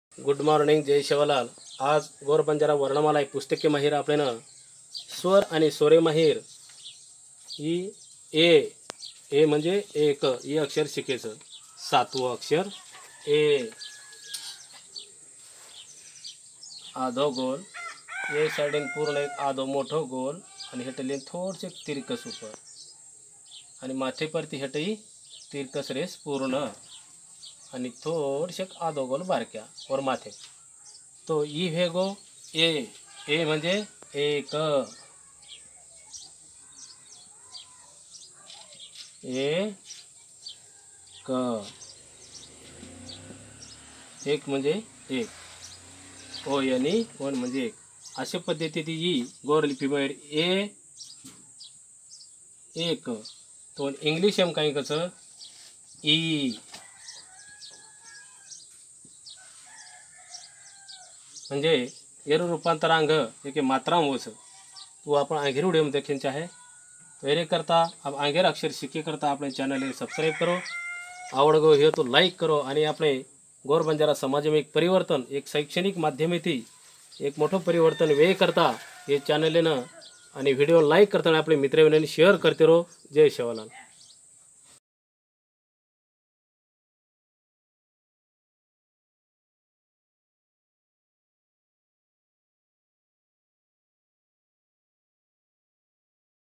How to learn and write "E" ए in gor banjara lipi and language. गोरबोली लिखे अब गोरबोली लिपिमे..
विश्वव्यापी गोरबंजारा शिक्षण अभियान भारत.गोरबोली भाषा लिखे अब गोरबोली लिपिमे.